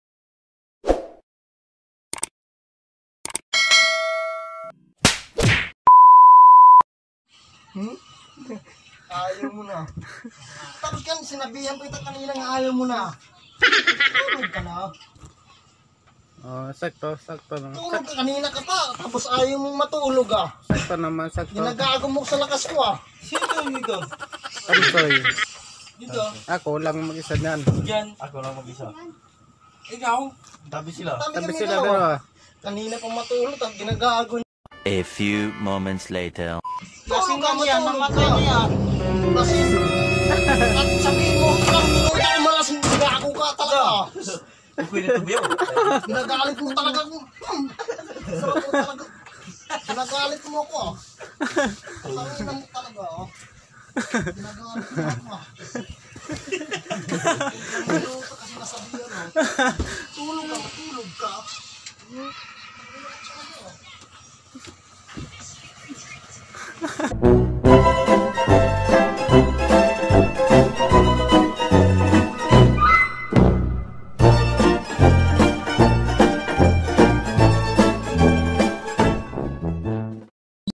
Epekto ng Emperador|Nagkapikonan
Nagkapikonan man natural lang yan, kailngan macontrol lang yung galit atleast kunti lang ang problema.Pero kami maya-maya o kinabukasan wala na yan